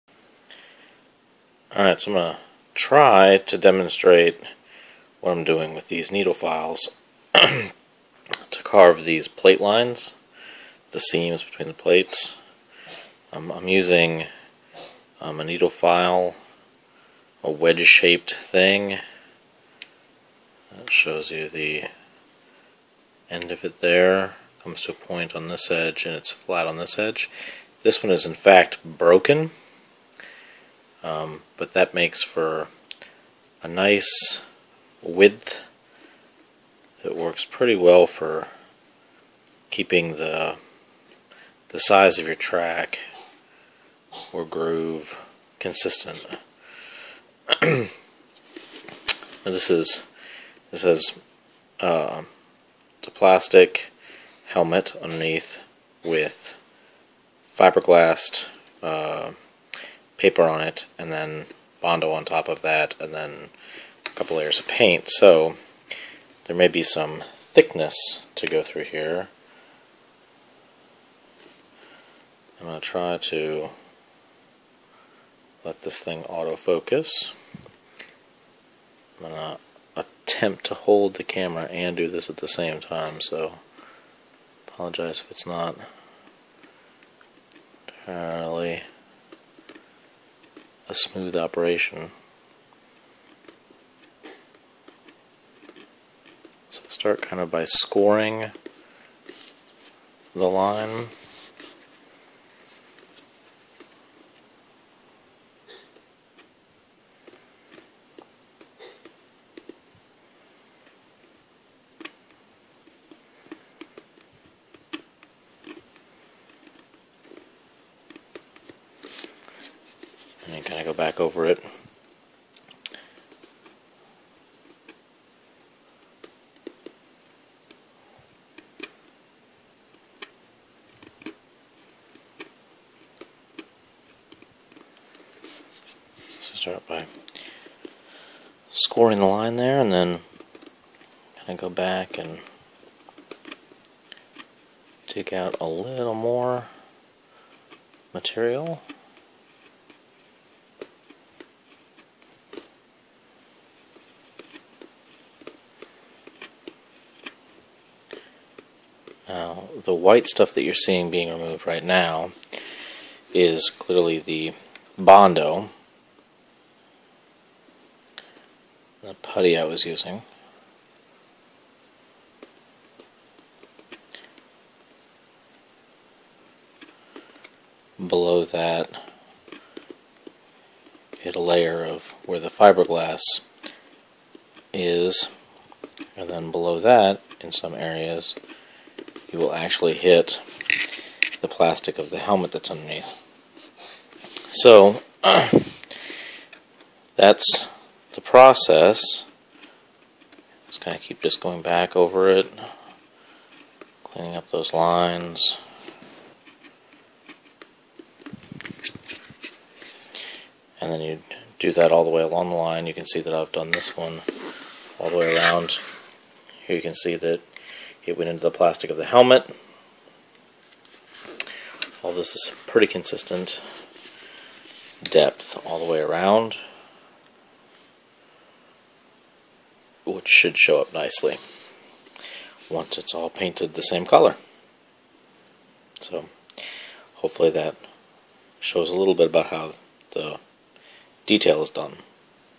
How to Create Detail Lines in a Cosplay Helmet
This is a quick video showing how I'm carving the detail lines into my pepakura helmet for a commissioned Ironman costume helmet I'm working on.  The full build thread on this costume can be found here: